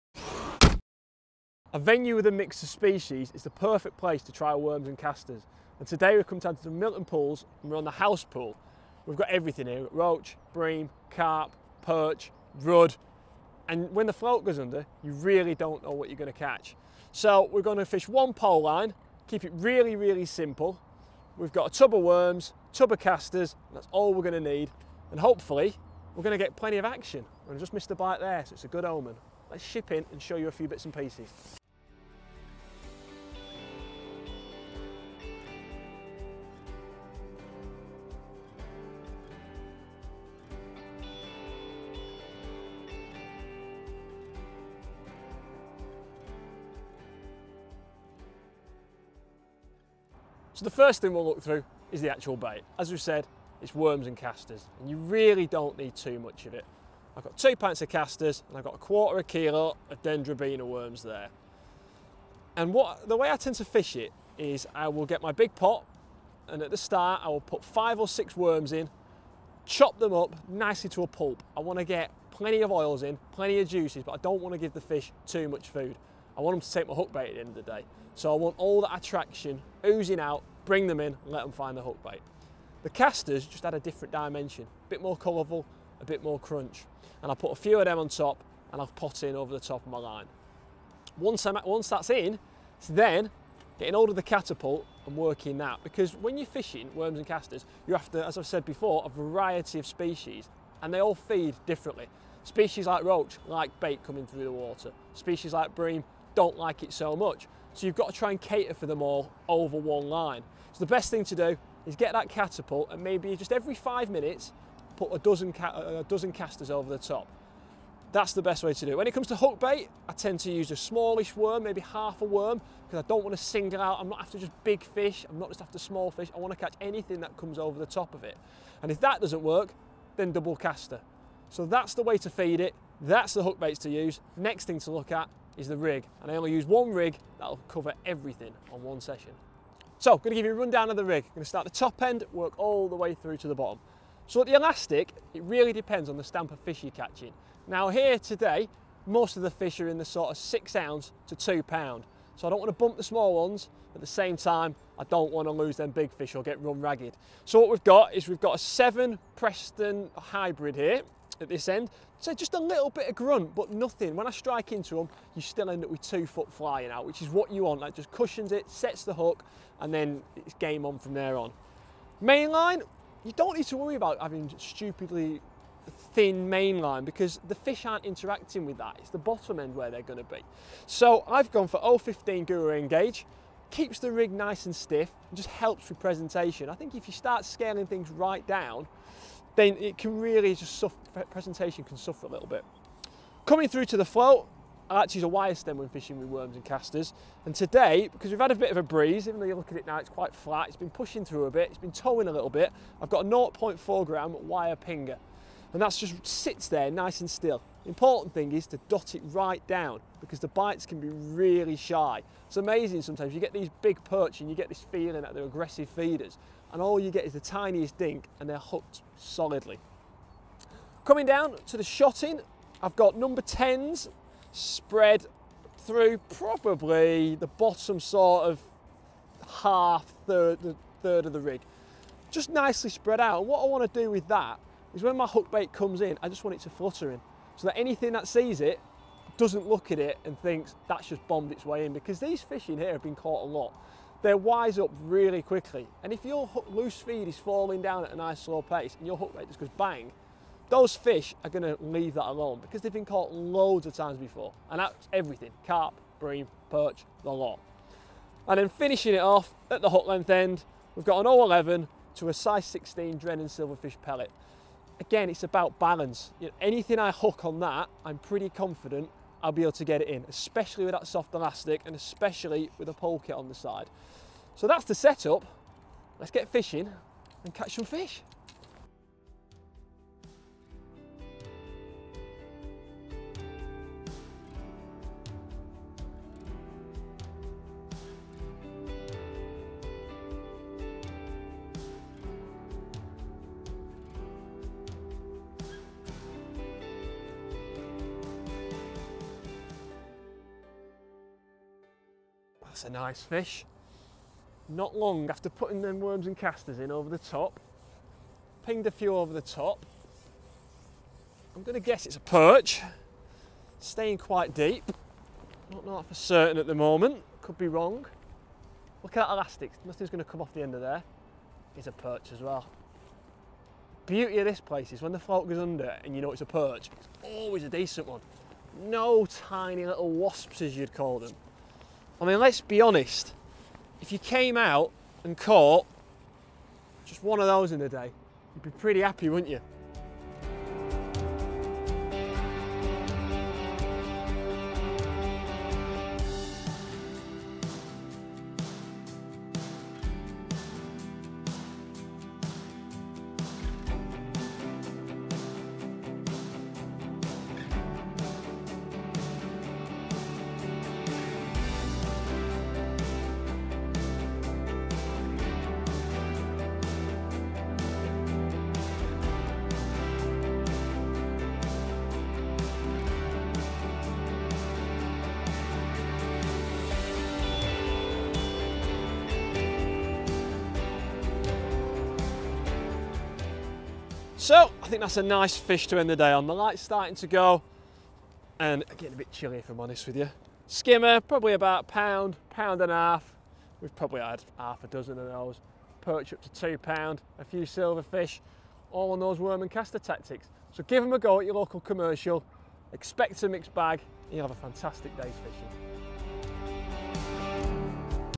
Get a bite every cast pole fishing...
Improve Your Coarse Fishing Features Editor Tony Grigorjevs heads to Milton Pool's House Pool for a pole fishing session using chopped worm and casters. This is a great method for catching plenty of fish on commercial venues, as Tony reveals!

Keep up to date with the latest fishing tackle by looking through our extensive selection of in-depth reviews.